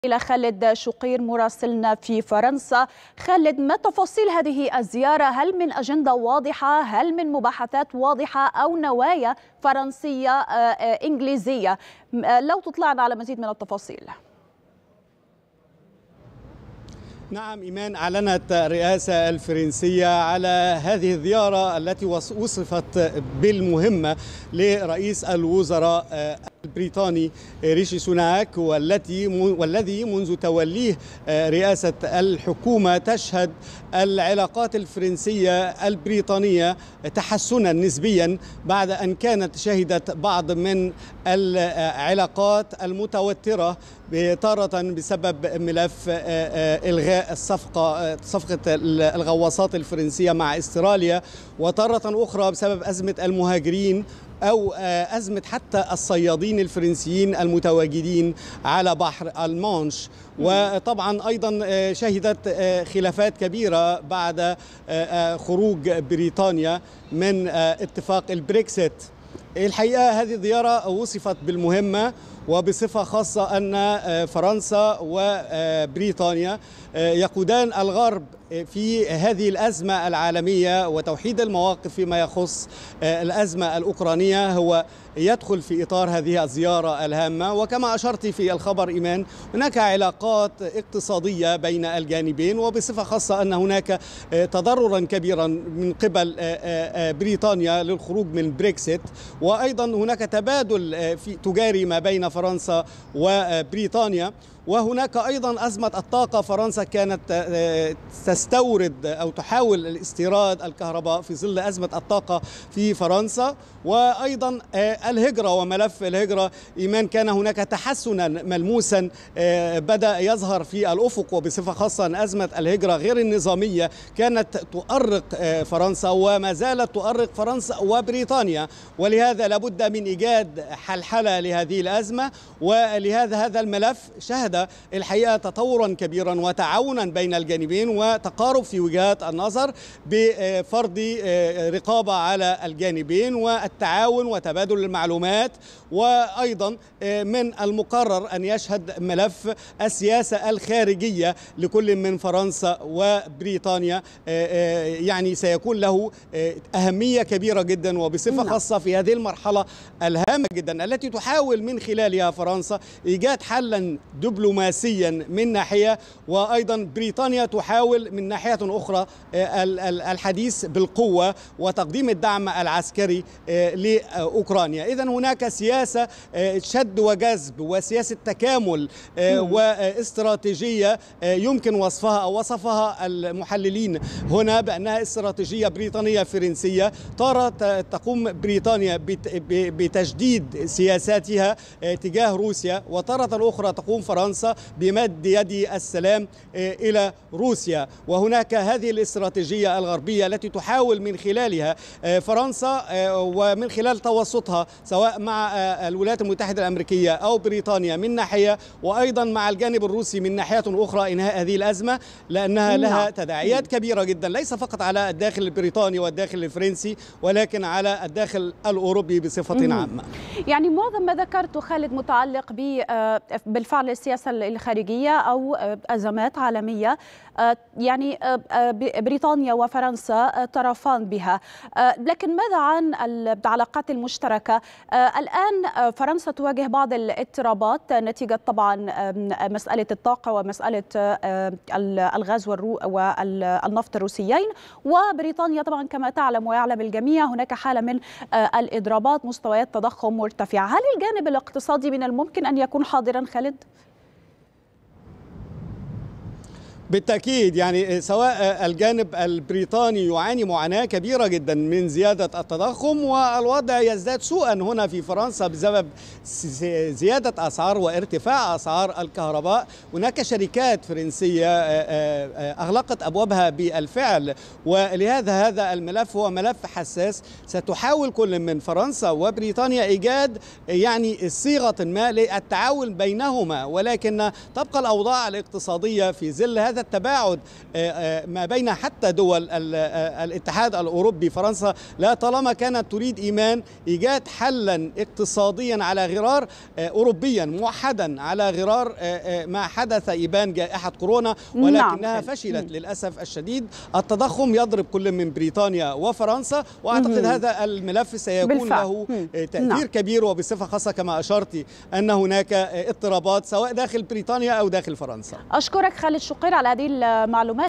ملفات مهمة على طاولة اللقاء المرتقب بين الرئيس الفرنسي ونظيره البريطاني| التفاصيل مع مراسلنا
ملفات مهمة على طاولة اللقاء المرتقب بين الرئيس الفرنسي ونظيره البريطاني| التفاصيل مع مراسلنا خالد شقير من مرسيليا

11/1/2023